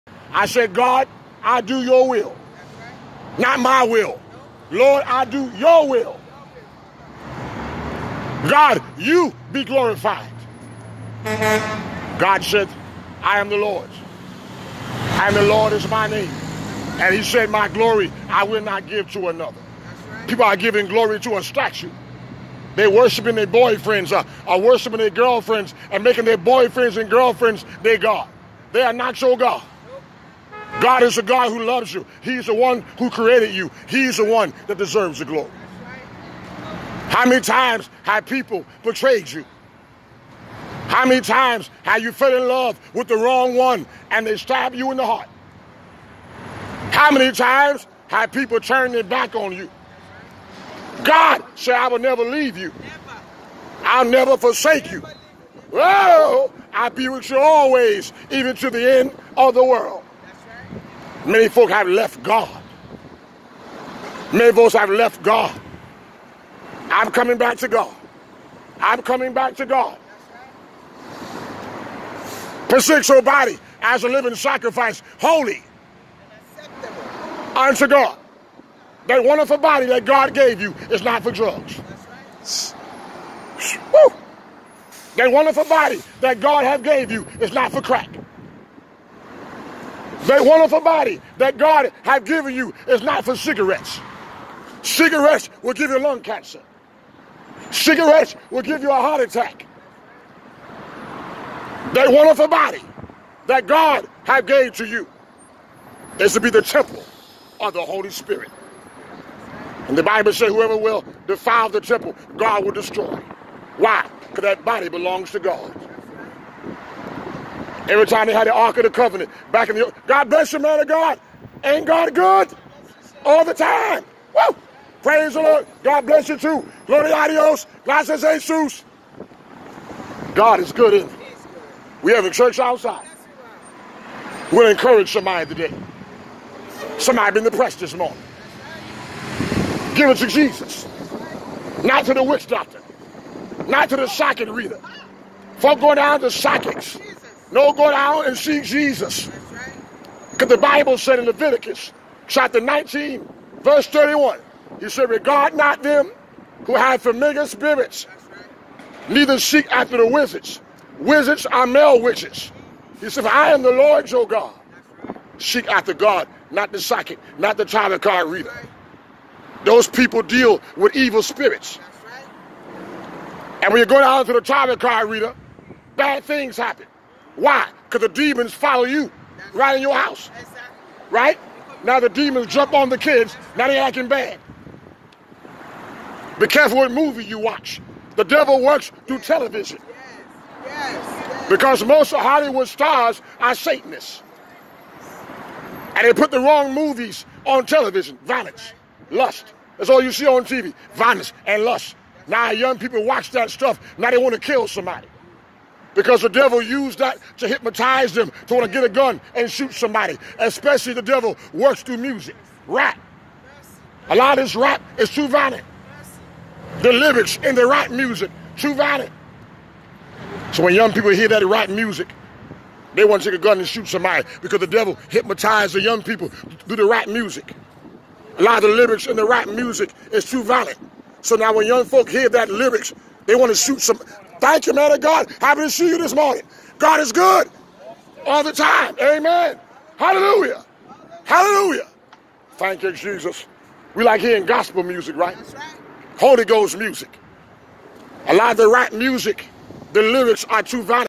LORD I WILL DO YOUR WILL! INSPIRATIONAL SERMON BY APOSTLE WARREN PREACHING ON FIRE FOR JESUS IN NJ!
HIGHLY ANOINTED VERY INSPIRATIONAL TRUTH BY APOSTLE WARREN DAVID ADAMS AKA PREACHER WARREN HALLELUJAH ENJOY!